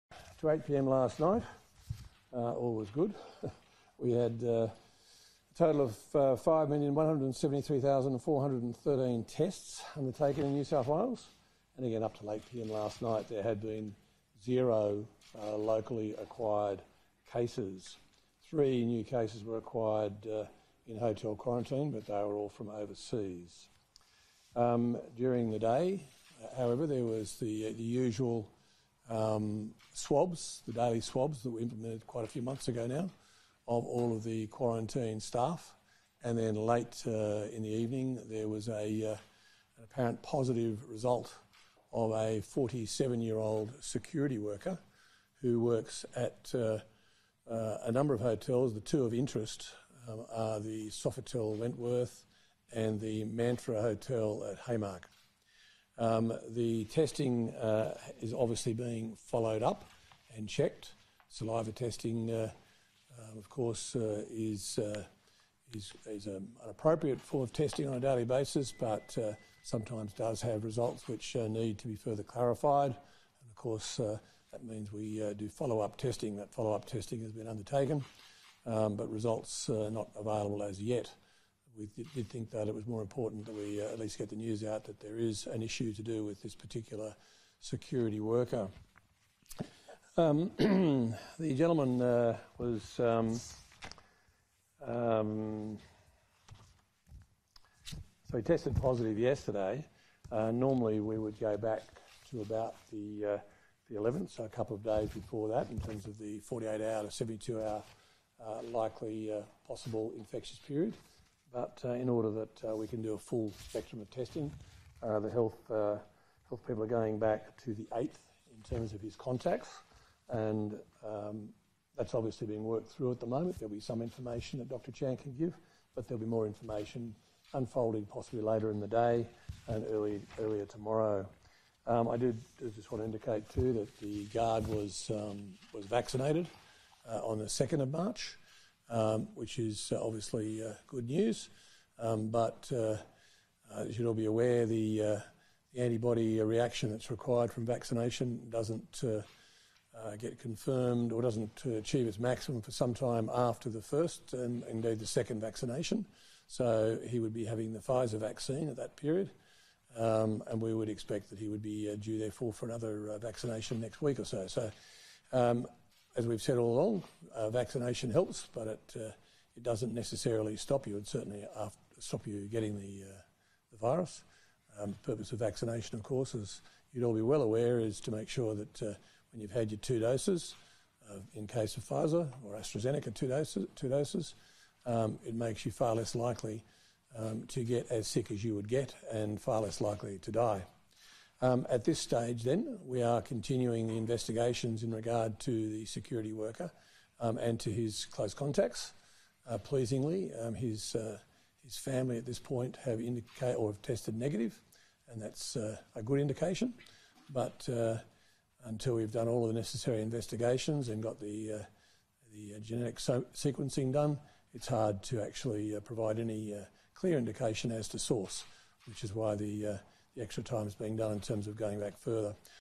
NSW Health confirms positive case
New South Wales Health Minister Brad Hazzard says the 47-year-old security worker who tested positive worked at a number of hotels.

Mr Hazzard said the two hotels of interest are the Sofitel Wentworth and the Mantra in Haymarket.

The source of the infection is under investigation with health authorities urgently tracing close contacts.